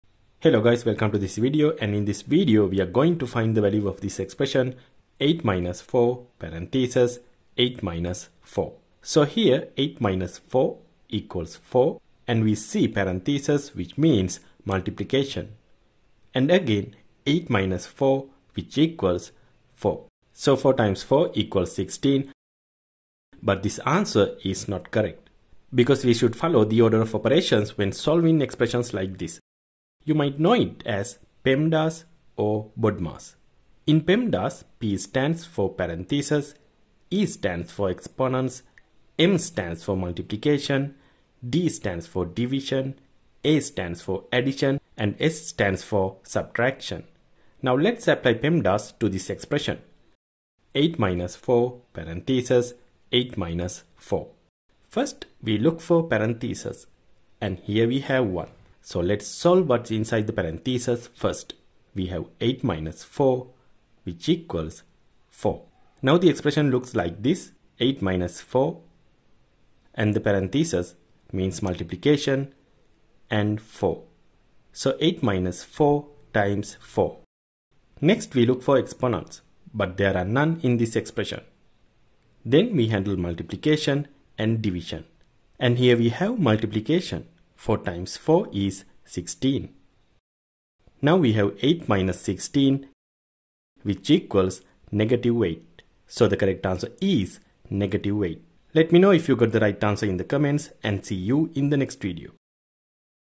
Most People Get This Simple Math Problem Wrong!
Think you're good at math? Let’s put your brain to the test with this quick expression: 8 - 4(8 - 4). Sounds easy, right? But most people get it wrong!

In this video, we’ll walk through the correct way to solve it using PEMDAS — the standard order of operations taught in schools. Whether you're a student brushing up for class or just love solving tricky math problems, this one's for you.

👉 We’ll break it down step by step, explain where most people go wrong, and show you the right way to think about it.
💬 Drop a comment with your answer before watching, and let us know if you nailed it or got tricked!

📌 Perfect for:

Students sharpening math skills
Parents helping kids with homework
Anyone who enjoys solving puzzles and brain teasers